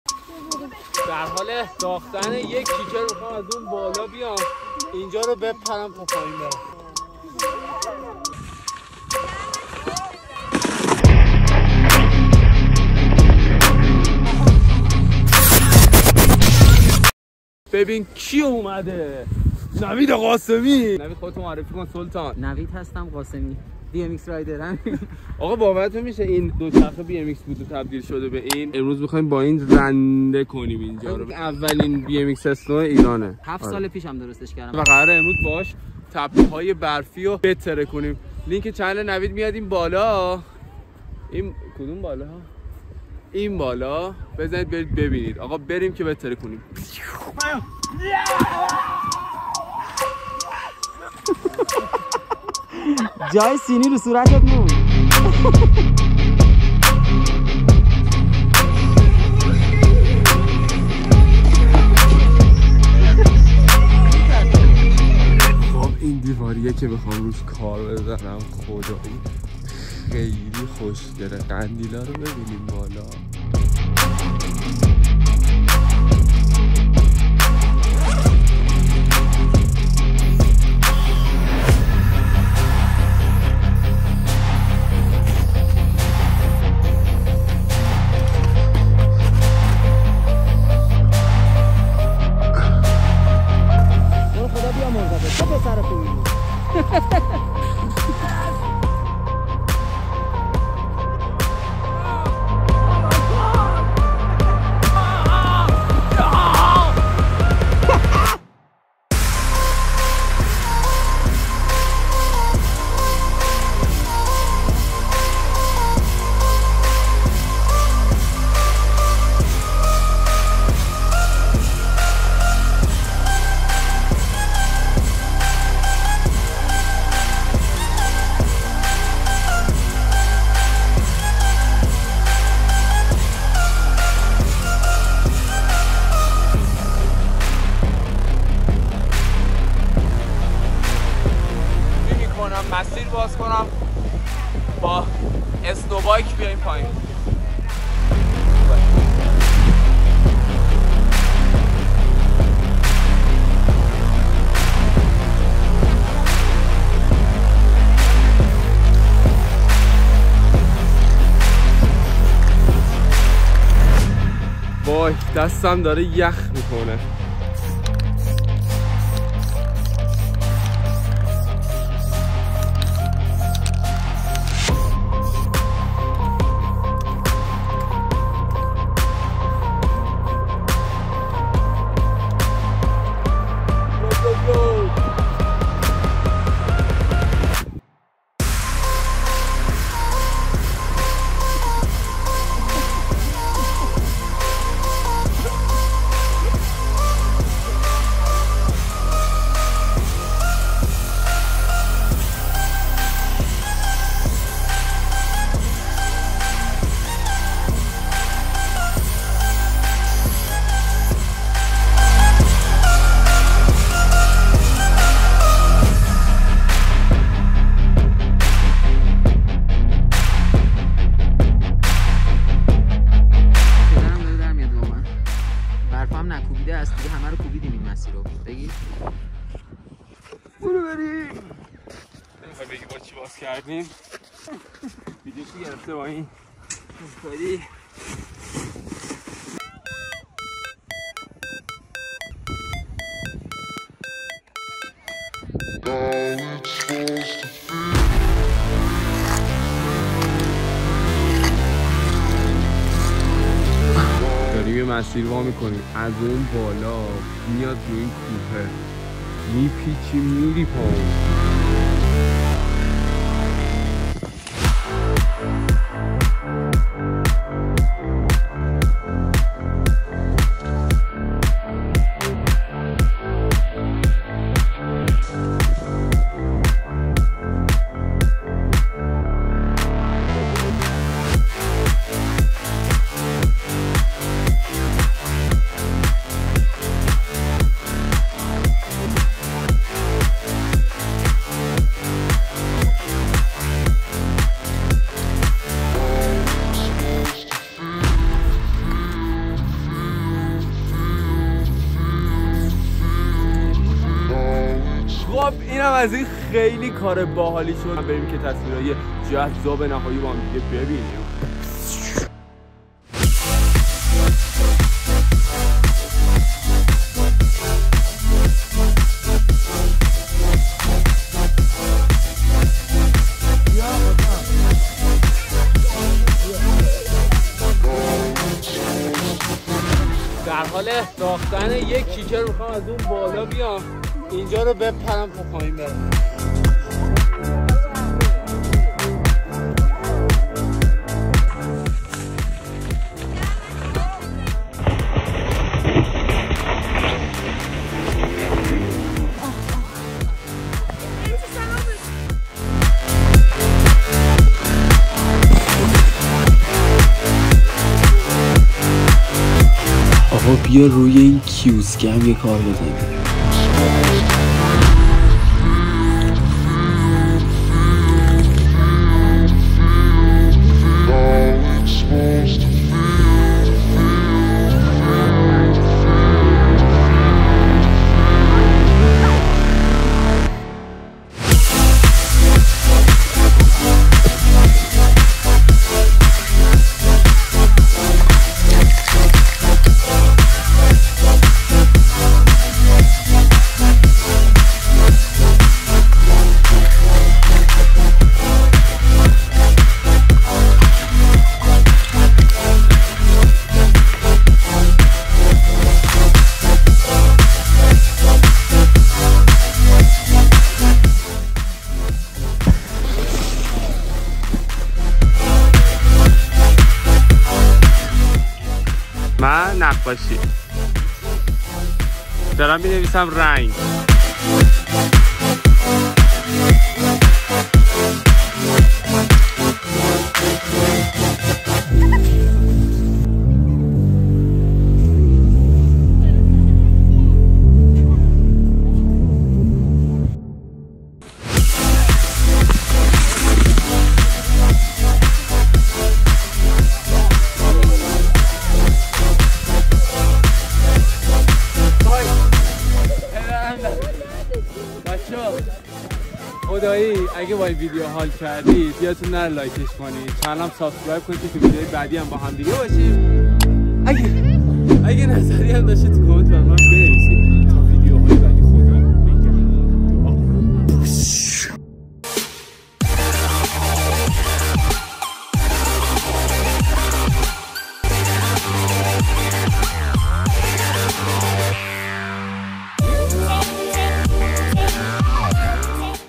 دوچرخ برفی ساختیم برای اولین بار تو ایران!
ما اومدیم برای اولین بار تو ایران اسنو بایک ساختیم یا همون دوچرخه برفی که توی این ولاگ میخوایم تستش کنیم
نتیجشو ببینی شوکه میشییی!
یه گرافیتی خفن هم میزنممم